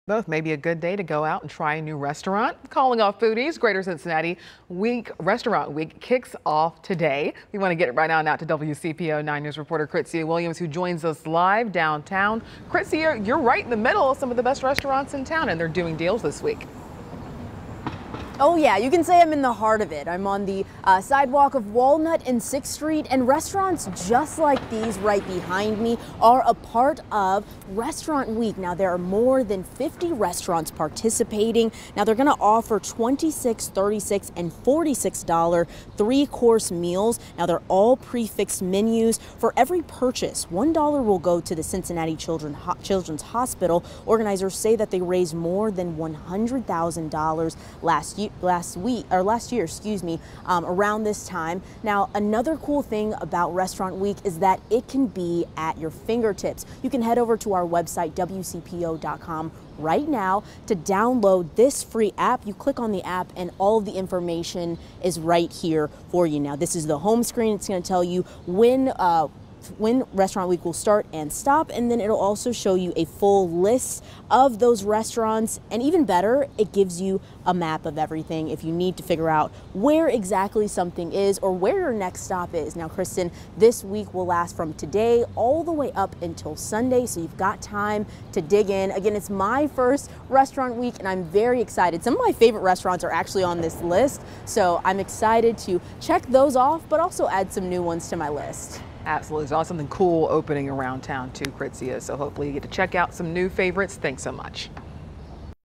Greater Cincinnati Restaurant Week starts Monday
The biannual restaurant week is offering $26, $36 and $46 three-course meals. For each meal eaten during the week, $1 will also be donated to Cincinnati Children's Hospital Medical Center.